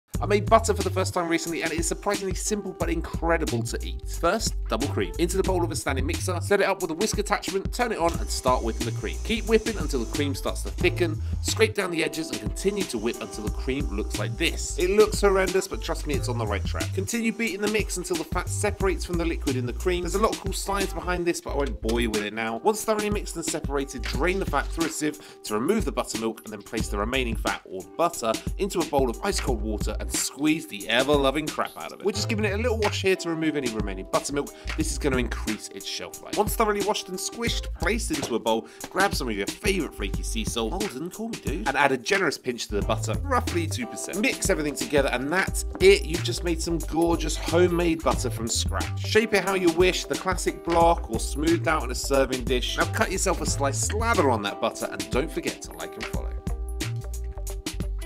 How To Make Your Own Butter
Homemade Butter Recipe:

1 litre double (heavy) cream
Sea salt

How To Make Your Own Butter | Homemade Butter